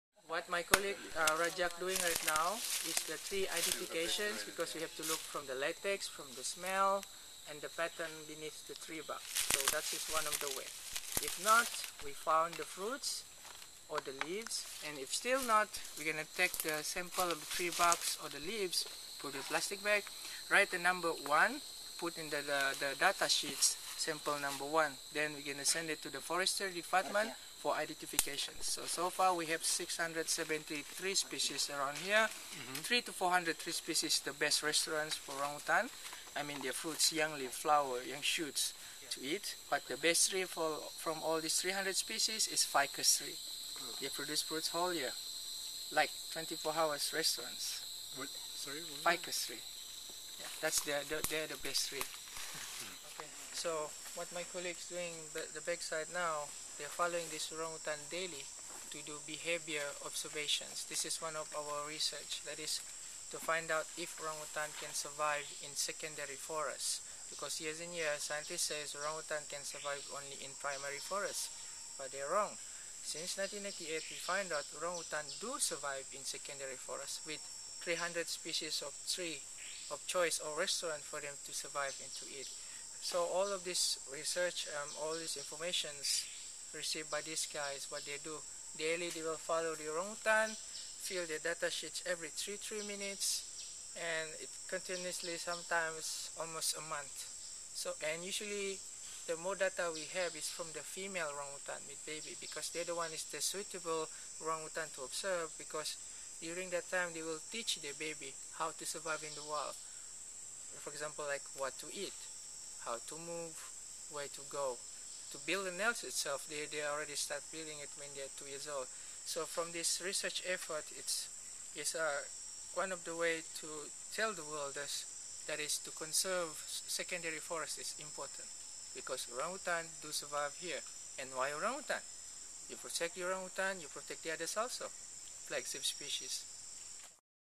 Red Ape Encounters Guide Explains Orangutan Observation in Sepilok Forest
During a Terra Incognita Ecotour, we enjoyed rare access to a protected site called Sepilok Forest. With a team of naturalists as our guides, we observed habituated wild orangutans.

In this video, Minchu of Red Ape Encounters describes a bit about the data they are collecting and how they determine which trees and fruits that orangutans prefer in the rainforest.